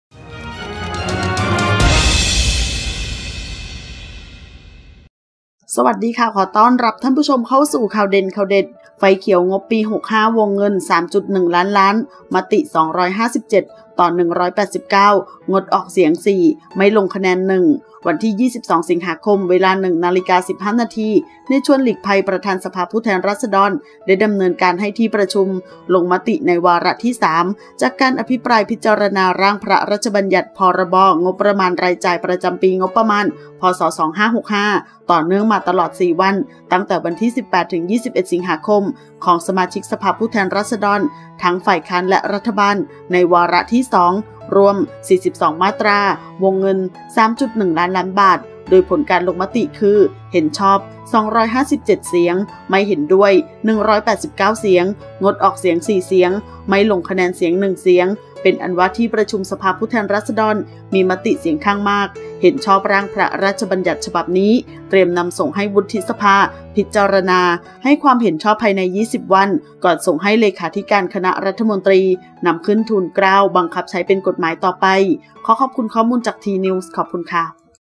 ไฟเขียว งบปี 65 วงเงิน 3.1 ล้านล้าน มติ 257 ต่อ 189 งดออกเสียง 4 ไม่ลงคะแนน 1
ขอบคุณข้อมูล:
ขอขอบคุณข้อมูลจาก: